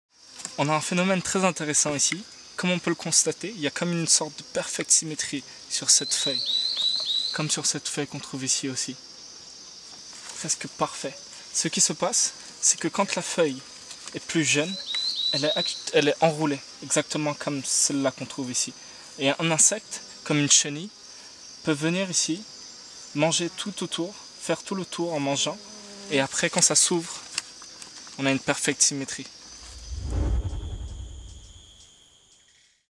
Les motifs mystérieux des feuilles en forêt équatoriale
Karl, spécialiste de la forêt tropicale, nous explique maintenant comment se forment les motifs composés de trous dans les feuilles.